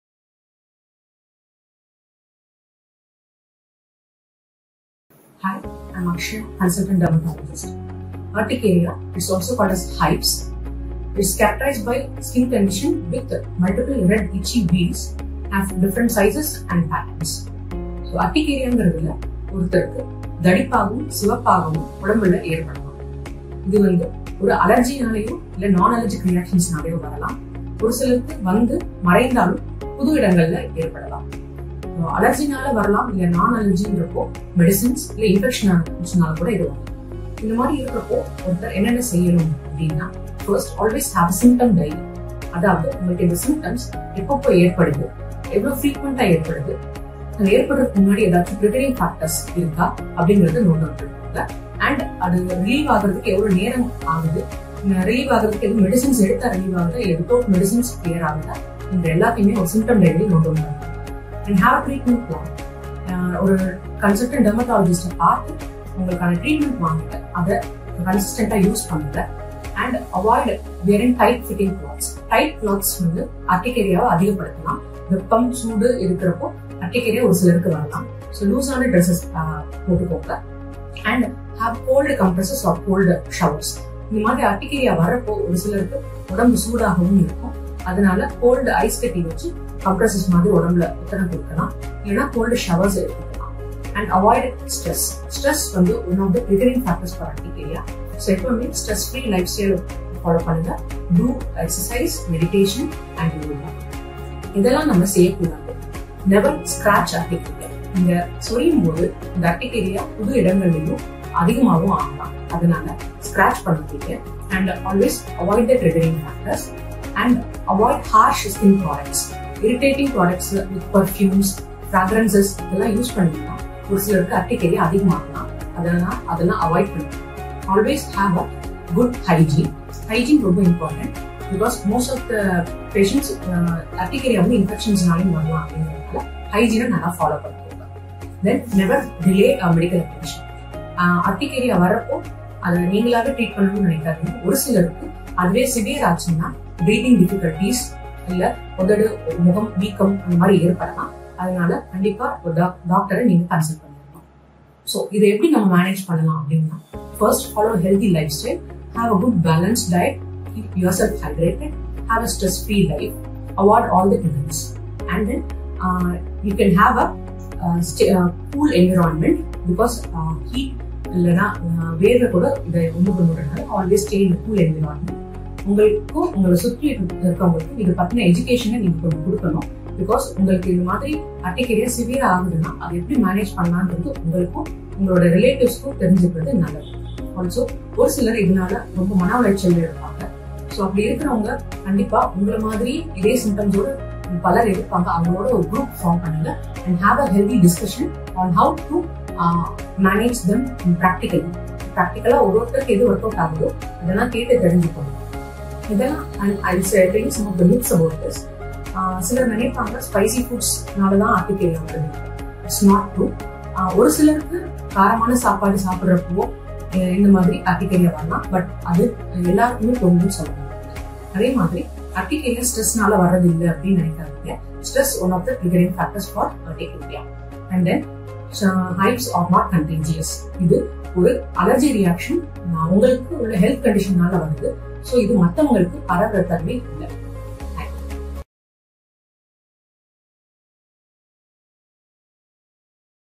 All about Urticaria : Dr Akshaya Sairam, Consultant Dermatologist (Tamil)
Welcome to the informative video by Dr.Akshaya Sairam, MBBS., MD (DVL) Consultant Dermatalogist. Here, Dr. Akshaya Sairam delves into the world of Urticaria, shedding light on its causes and the various factors that can trigger this condition. With a focus on practical advice, She shares essential Do's and Don'ts to help you manage Urticaria effectively. Our goal is to empower you with valuable tips and insights that can make a difference in your daily life, ensuring that Urticaria doesn't interfere with your well-being. Join us on this journey to better understand and combat Urticaria!